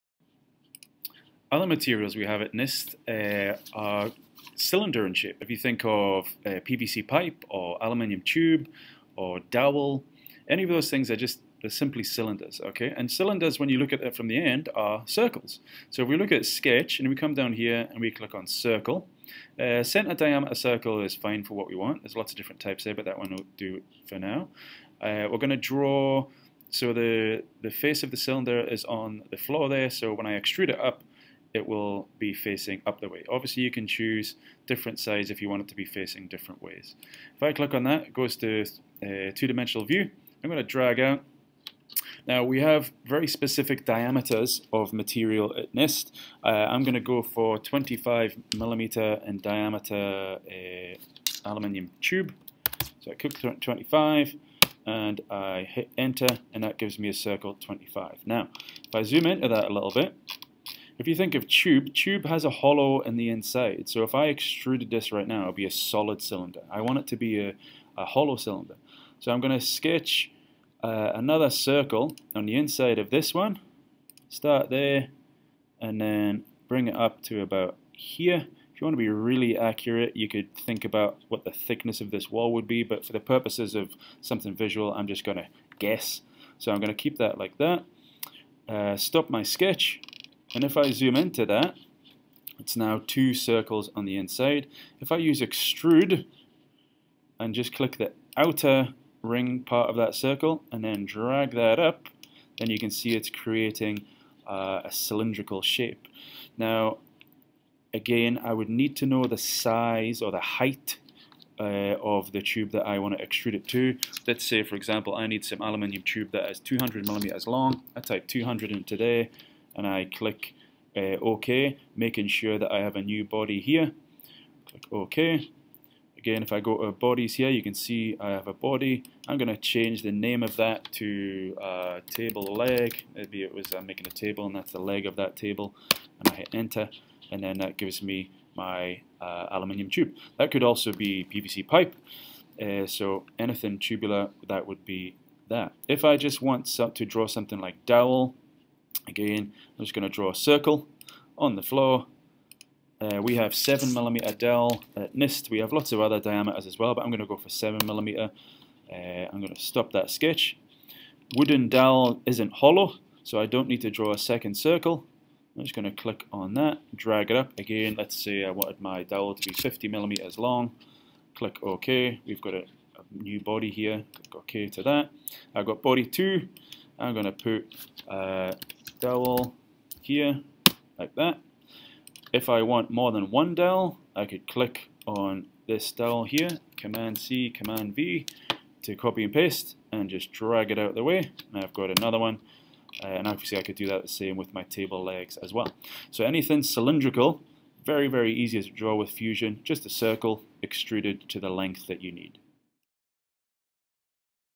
3D CAD with Fusion 360 - Drawing PVC Pipe, Aluminium Tube and Dowel
Learn how to draw cylinders.